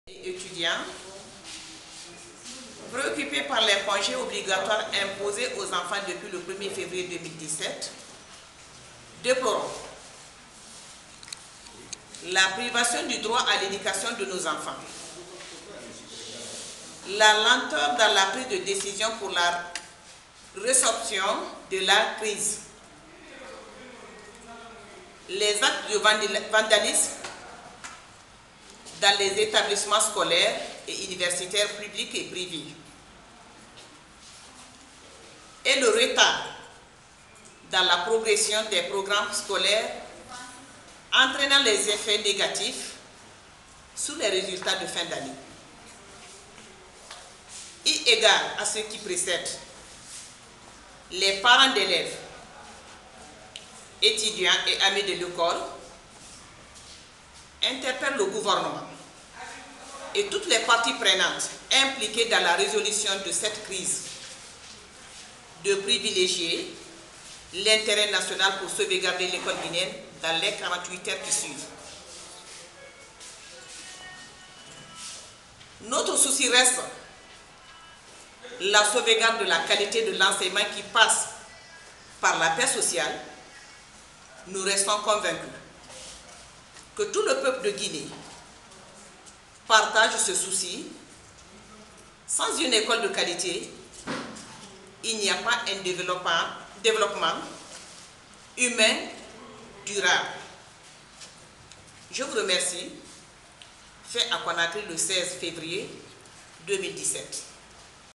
Fermeture des écoles, instituts et universités en Guinée : les parents d'élèves dénoncent
Depuis le 3 février, le Gouvernent guinéen a imposé un congé forcé aux élèves et étudiants des écoles, instituts et universités sur toute l'étendue du territoire guinéen. Confrontées à une grève des enseignants du secteur publics, les autorités font actuellement chômer à la fois le public et le privé. Frustrés de voir chaque jour les enfants à la maison, les parents d'élèves ont convié les médias à une déclaration hier, jeudi 16 février 2017 pour dénoncer cette façon de gouverner notre pays et exiger que l'Etat fasse le nécessaire pour satisfaire les revendications syndicales et permettre aux élèves et étudiants de reprendre le chemin de l'école.